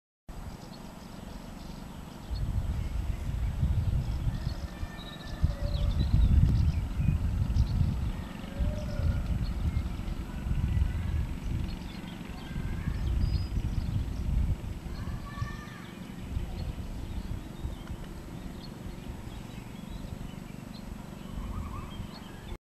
Coypu eating, close up from rear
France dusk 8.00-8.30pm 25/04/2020